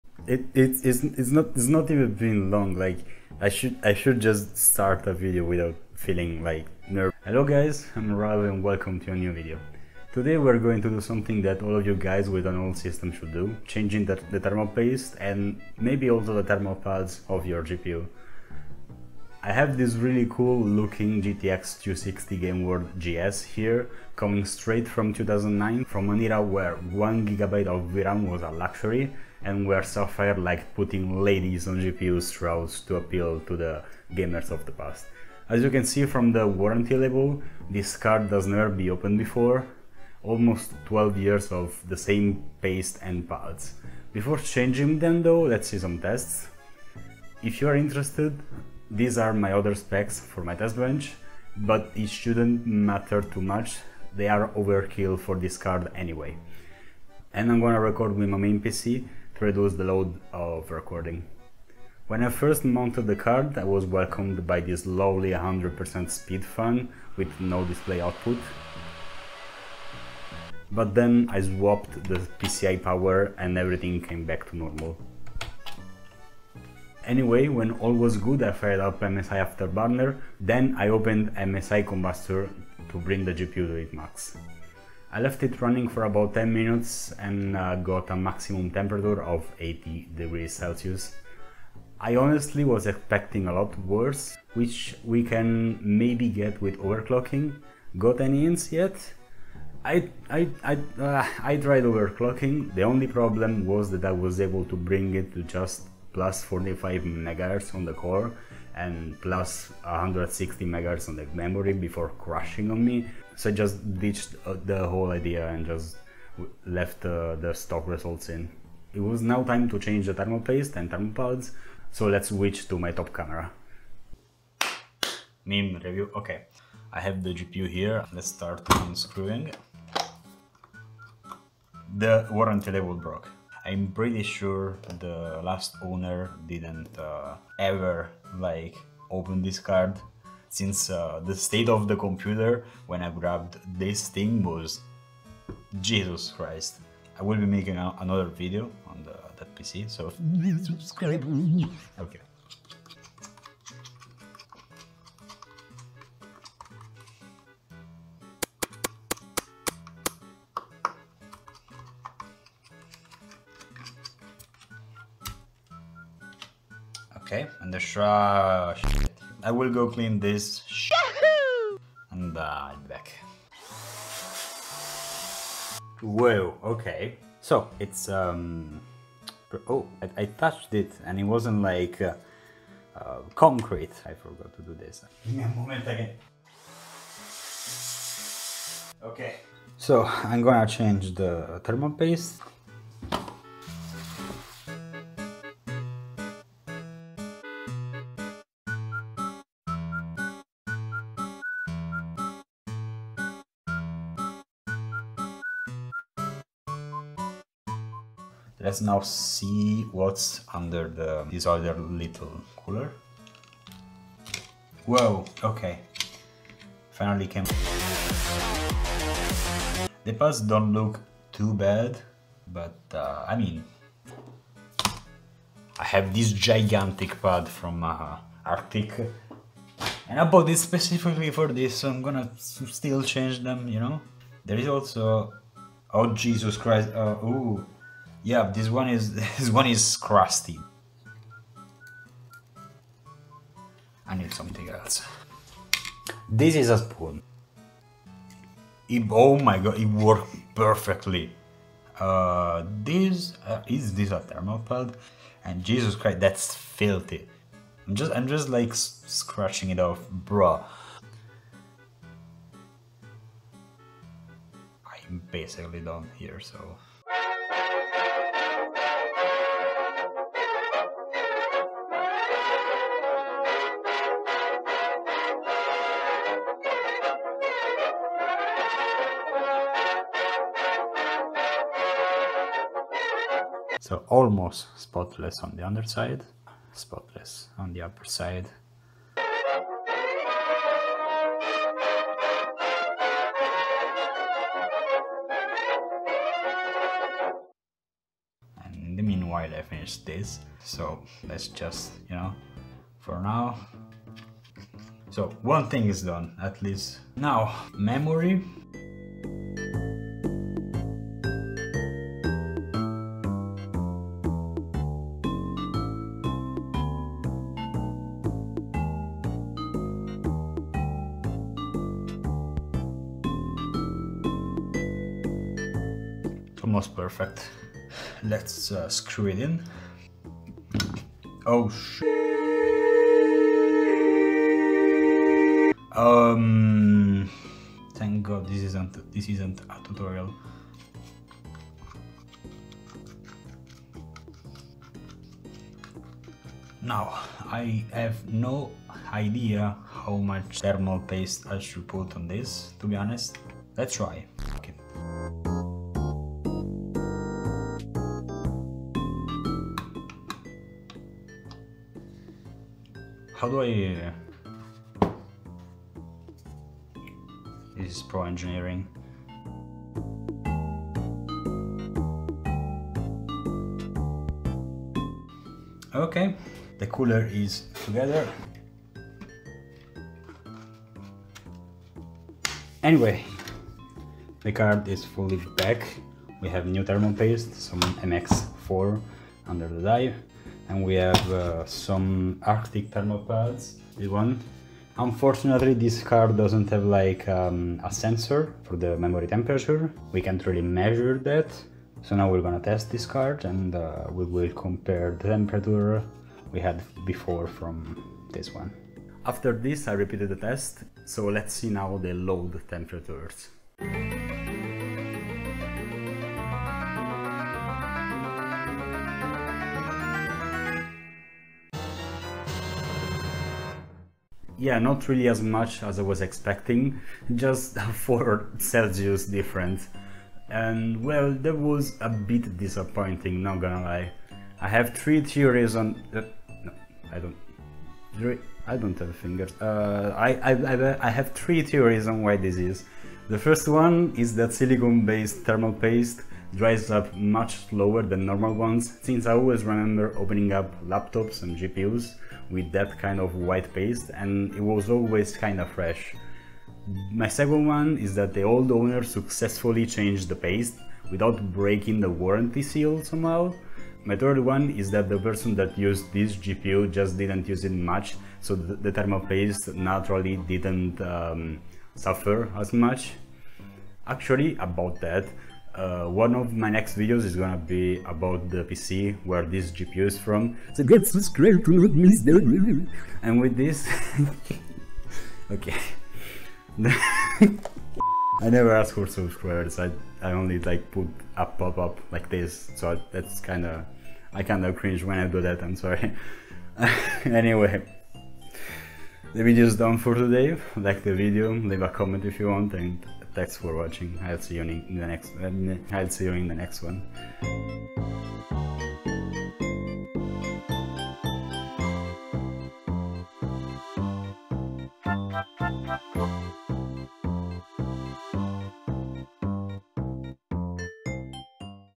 Replacing a GPU's 12 YEARS OLD Thermal Paste
Finally this GTX 260 is getting new thermal paste and thermal pads, but will they be enough to bring the maximum temperature down at least a bit? Stick around to the end to find out!

Timestamps:
Intro - 00:00
Testbench Specs - 00:46
Mounting the GPU - 01:01
MSI Kombustor [Old thermal paste] - 01:16
Overclocking??? - 01:31
Disassembly - 02:03
Changing the Pads - 03:39
Thermal Paste Time - 05:46
Reassembly - 06:00
MSI Kombustor [New thermal paste] - 06:48
Next videos, ducking around and Outro- 07:53